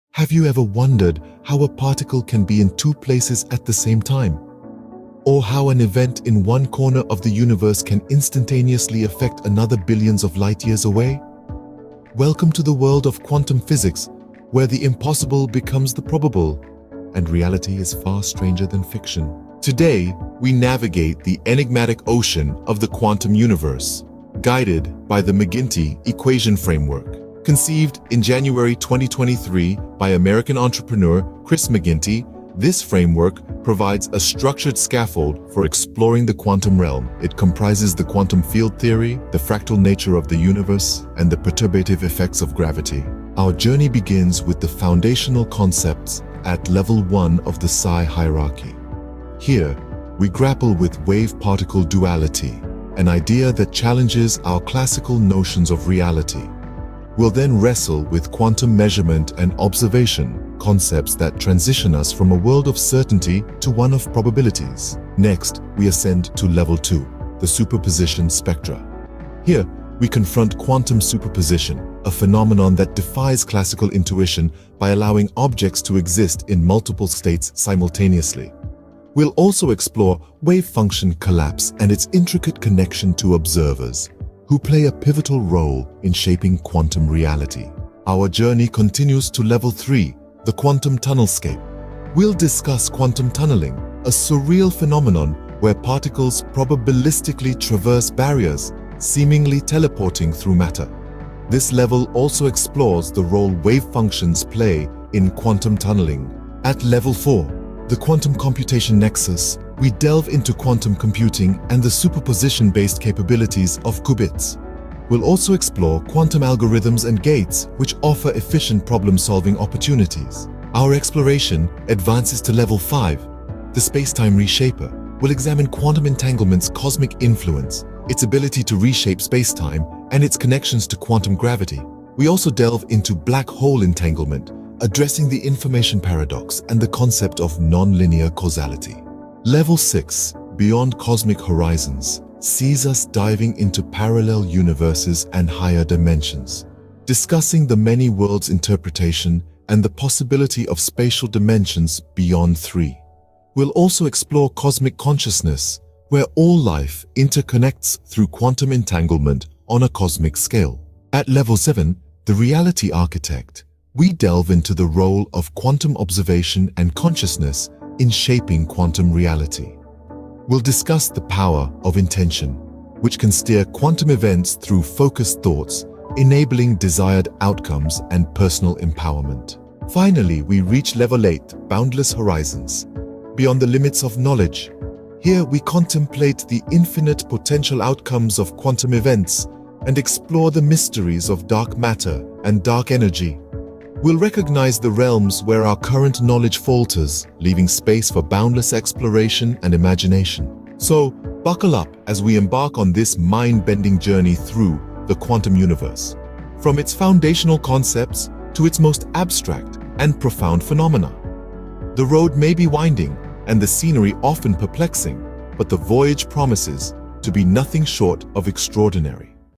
Navigating the Enigmatic Ψ Hierarchy
A captivating expedition into the mind-bending world of quantum physics, where reality takes on surreal dimensions, and the boundaries of the possible are pushed to the extreme. This video serves as a guide through the enigmatic quantum cosmos, offering viewers a structured framework for navigating the complex and intriguing realm of quantum physics.

The journey commences with a deep dive into the foundational concepts of quantum physics at level one of the Ψ Hierarchy. Here, viewers grapple with the mind-boggling notion of wave-particle duality, where particles exhibit both wave-like and particle-like properties, challenging our classical understanding of reality. The exploration then takes a fascinating turn as it delves into the mysteries of quantum measurement and observation, concepts that transition us from a deterministic world to a realm of probabilities.

Advancing to level two, the superposition spectra, viewers are confronted with the concept of quantum superposition, where objects can exist in multiple states simultaneously. This phenomenon defies classical intuition and leads us to explore the intricacies of wave function collapse. The profound connection between wave function collapse and the role of observers in shaping quantum reality becomes apparent, prompting deep philosophical contemplation.

The journey continues to level three, the quantum tunnel scape, where the surreal phenomenon of quantum tunneling is unveiled. Here, particles probabilistically traverse barriers, seemingly teleporting through matter. The role of wave functions in facilitating quantum tunneling is also explored, adding depth to our understanding of this phenomenon.

At level four, the quantum computation Nexus, viewers are introduced to the exciting world of quantum computing. They learn about qubits, the fundamental units of quantum information, and delve into quantum algorithms and gates, offering efficient problem-solving opportunities that have the potential to revolutionize various fields.

The expedition advances to level five, the space-time reshaper, where the concept of quantum entanglement takes center stage. Viewers gain insights into how quantum entanglement can reshape space-time and its connections to quantum gravity. The entanglement of black holes, the information paradox, and nonlinear causality are explored, revealing the profound implications of this phenomenon.

Level six, Beyond Cosmic Horizons, takes viewers on a journey into the realms of parallel universes and higher dimensions. The many worlds interpretation and the possibility of spatial dimensions beyond three are discussed, igniting the imagination. Cosmic consciousness, where all life interconnects through quantum entanglement on a cosmic scale, is another captivating topic explored.

The voyage continues to level seven, the reality architect, where the role of quantum observation and consciousness in shaping quantum reality is contemplated. The power of intention, harnessed through focused thoughts, is discussed, showcasing the potential for personal empowerment through quantum events.

Finally, the expedition culminates at level eight, boundless horizons, where viewers contemplate the infinite potential outcomes of quantum events. The mysteries of dark matter and dark energy are pondered, recognizing that there are realms where our current knowledge falters, leaving space for boundless exploration and imagination.

In this mind-bending journey through the quantum universe, from its foundational concepts to its most abstract and profound phenomena, viewers are in for an extraordinary and thought-provoking experience. The road may be winding, and the scenery often perplexing, but the voyage promises to expand horizons and challenge conventional understanding in the most extraordinary ways. Buckle up for an exploration of the quantum universe like no other.